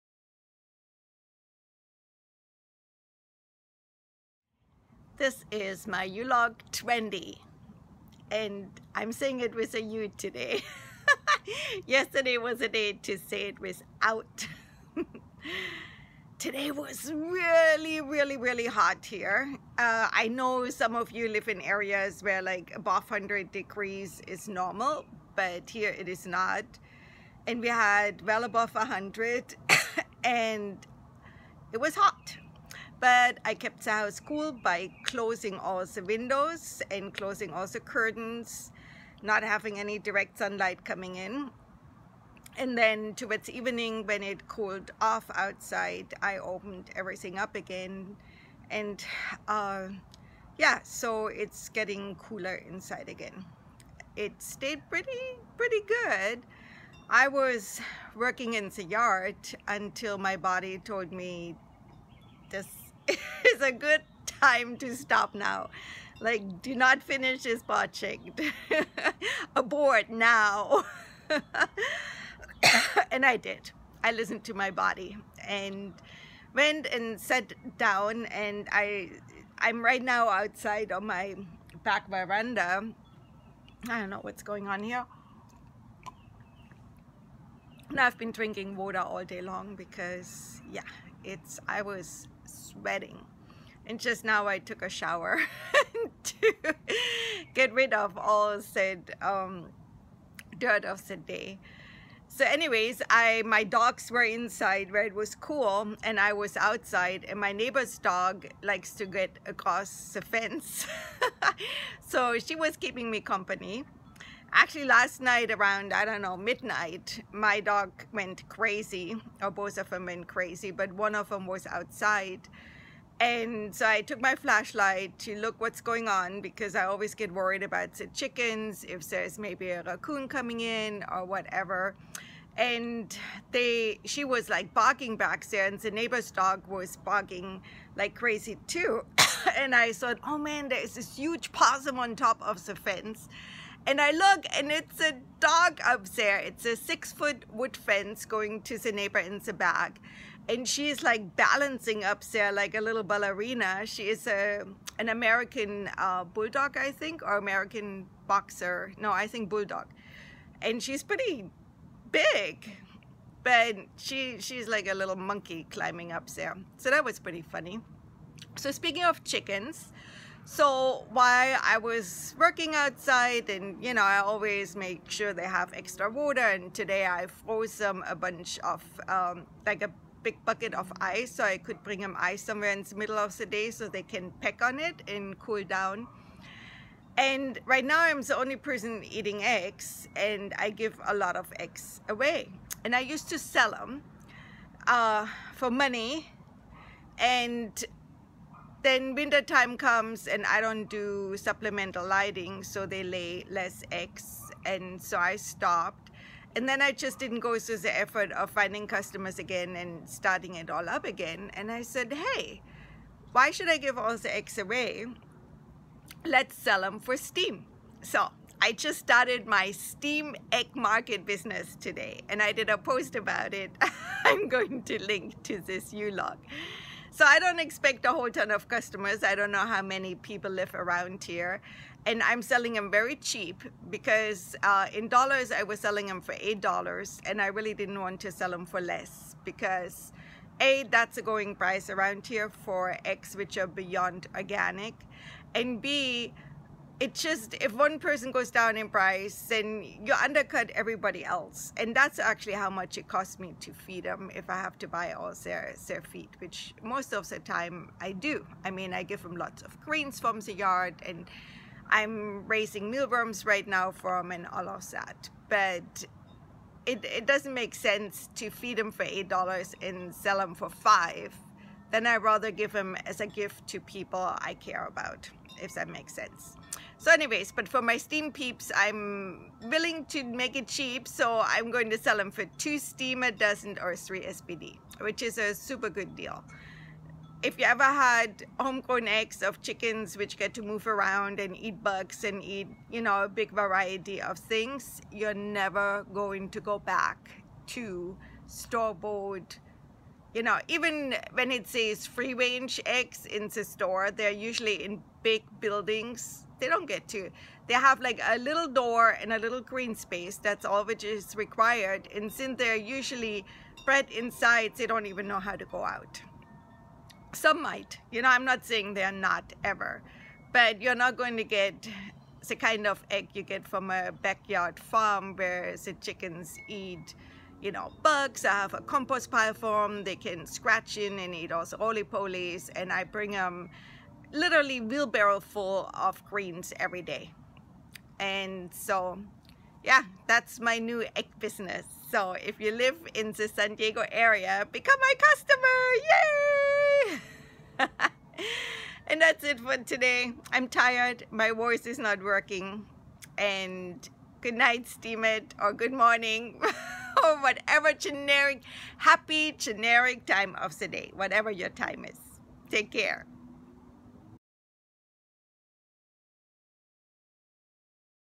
Ulog 20 Steem Egg Business
While working in the garden on a very hot day, I got the idea to sell eggs for a cryptocurrency called Steem.
I will make them cheap for those who want to pay in Steem :)
Here is a blog post about that.